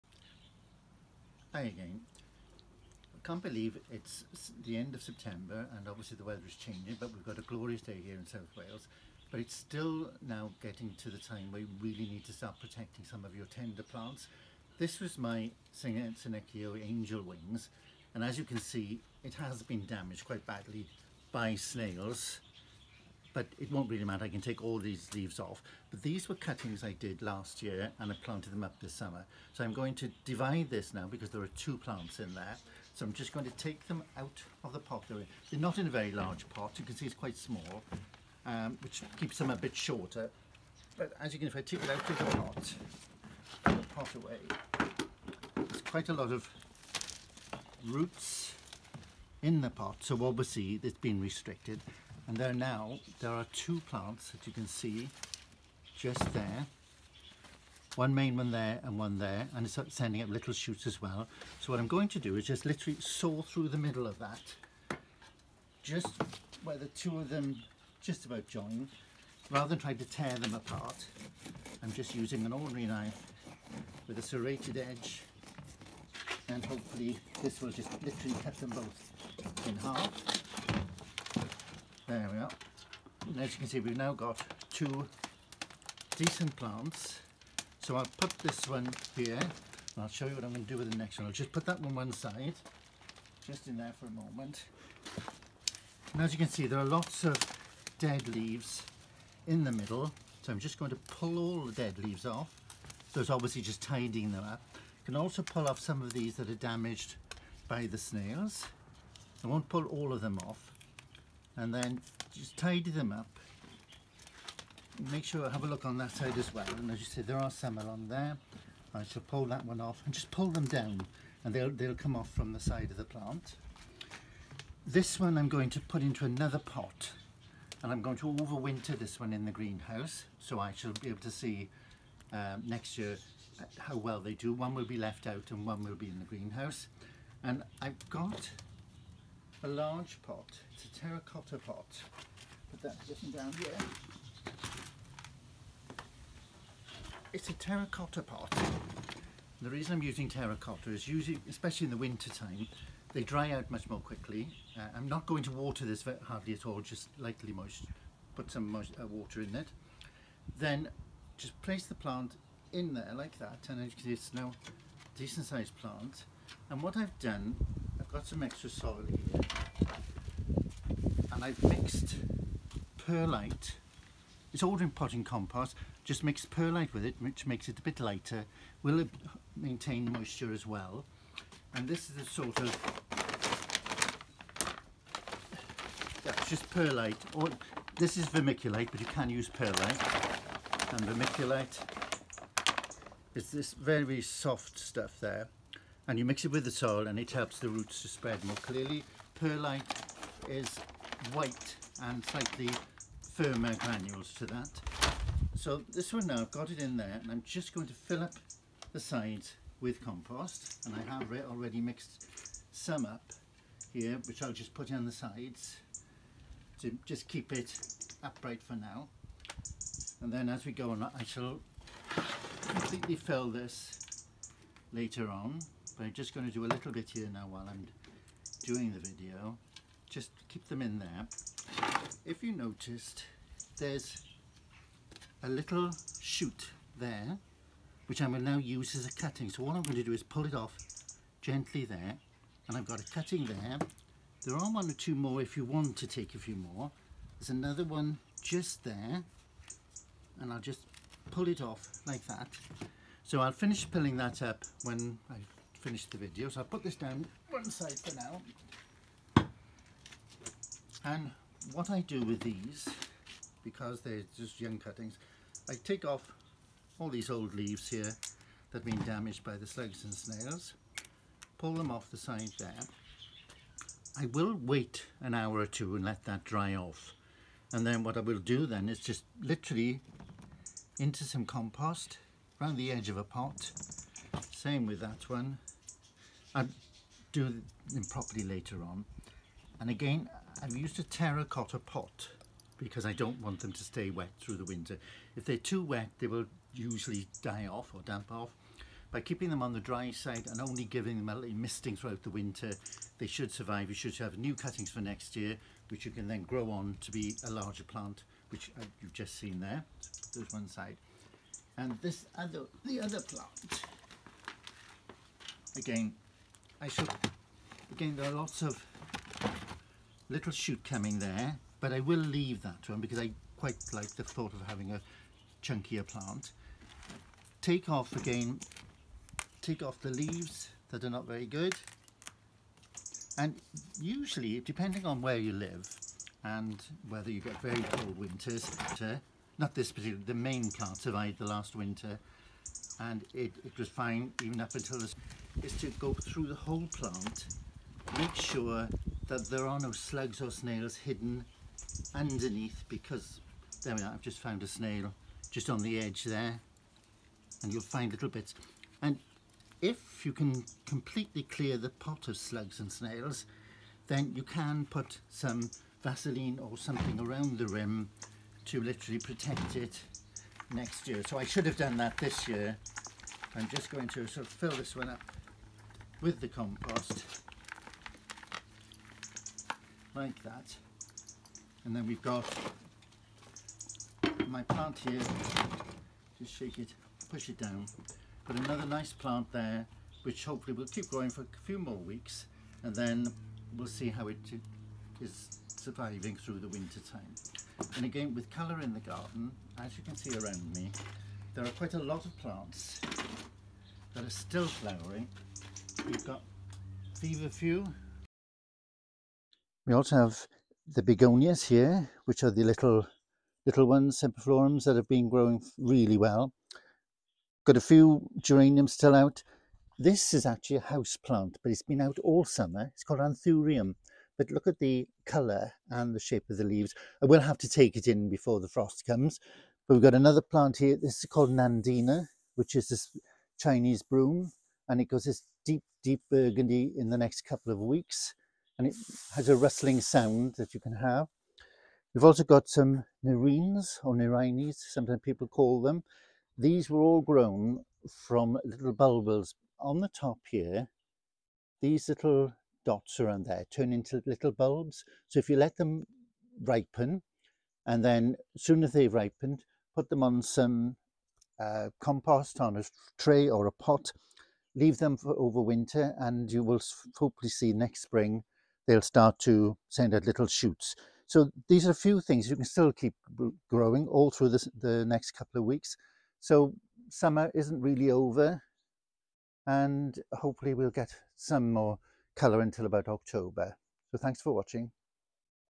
Angel Wings saving my plants over winter
This is how I take Cuttings from my Angel Wings and save the parent plant during winter. I know there are many different thoughts about this plant but this is how I manage my plant.